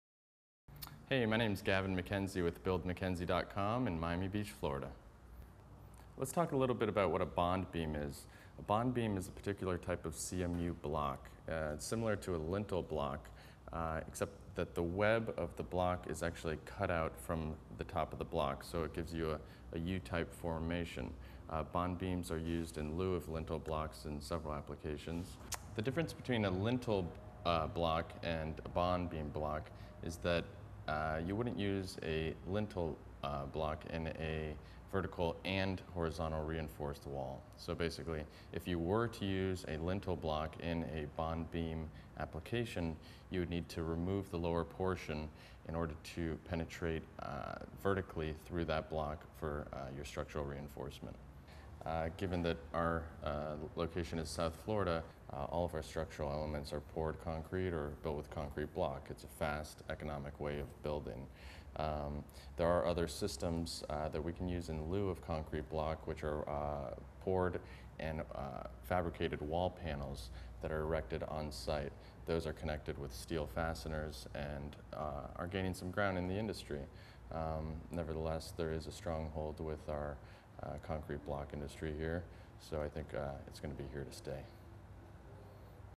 The Construction Business : What Is a Bond Beam?
A bond beam is a particular type of CMU block with the web of the block being cut out from the top to give a U-type formation. Learn more about bond beams from a general contractor in this free video on bond beams.

Bio: Gavin McKenzie graduated from the University of Florida in 2003 with a degree in business administration and is the owner of McKenzie Construction L.L.C., in South Florida.